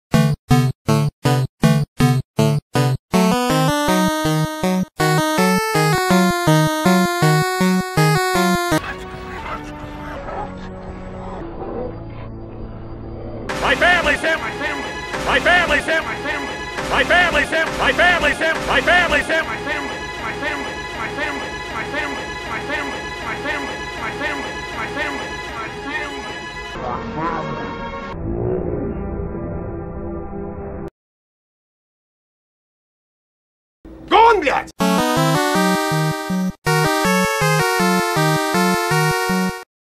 MQJ Shorts: Harrison Ford Says "My Family!" A Bunch of Times
My family! What about my FAMILY! Not without my FAMILY!